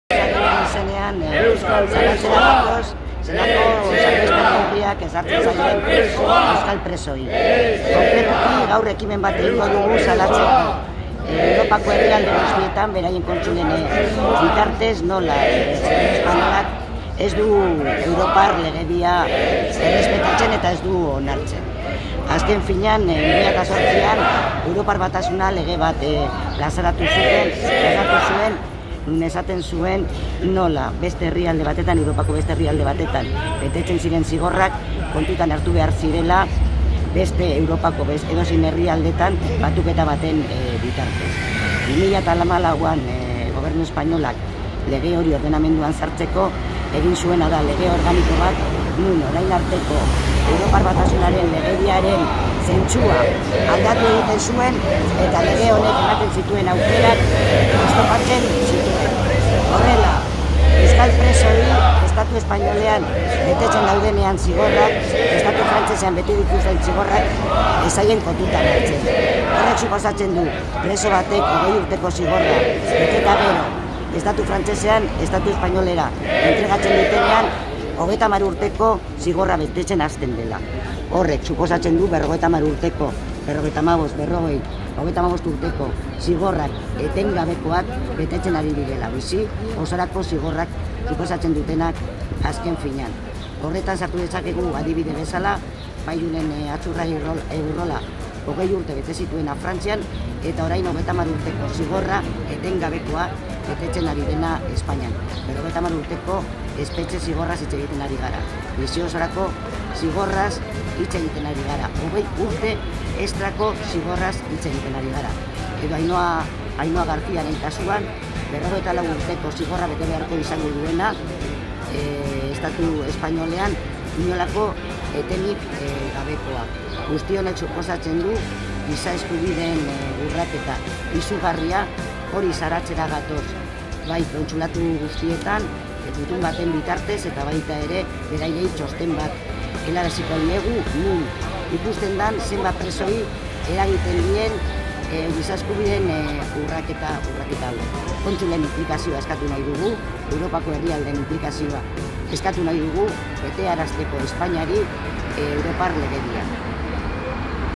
Europako gobernuei gutuna helarazi diegu, kondena bikoitza jasotzen duten presoen egoera azalduz
Italia,Frantzia, Herbereak, Belgika, Australia, Finlandia eta Alemaniako kontsul-etxeetan gutuna entregatu genuen atzo Bilboko giza katean.

Europako diplomazialariri helarazi diogu Euskal Presoei ezartzen zaien Kondena bikoitzaren inguruko txostena